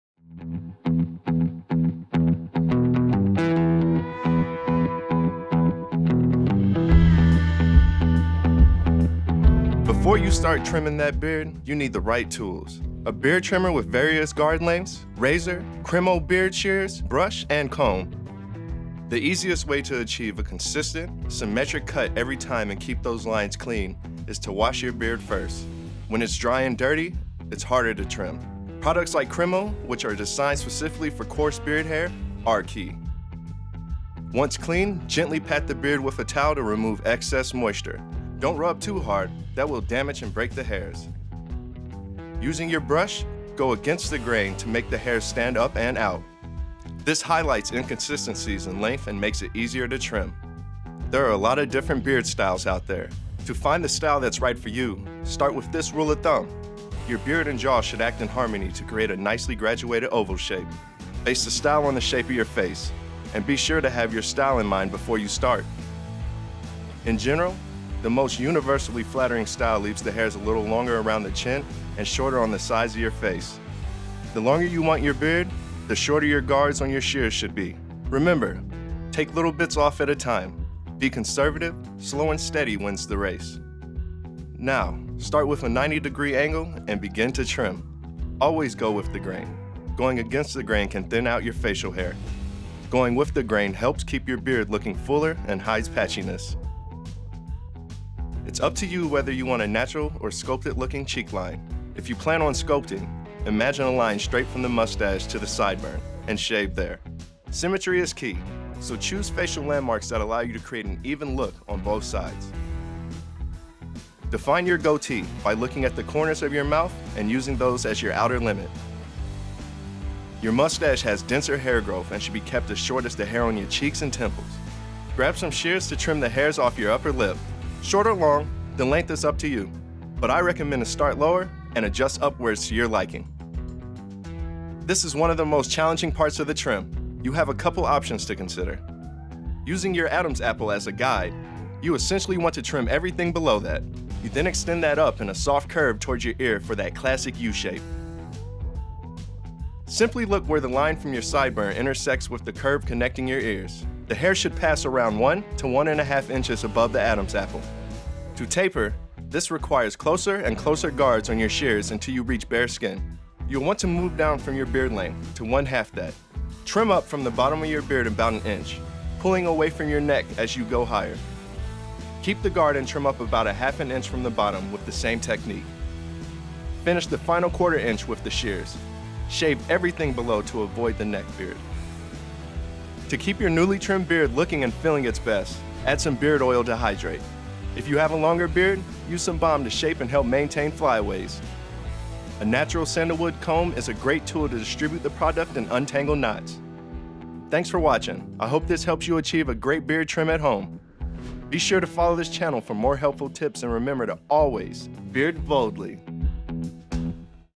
How to Shape Your Beard: Trimming and Cutting Techniques
See how to bring the barbershop beard trim home with a few tools and these easy tips.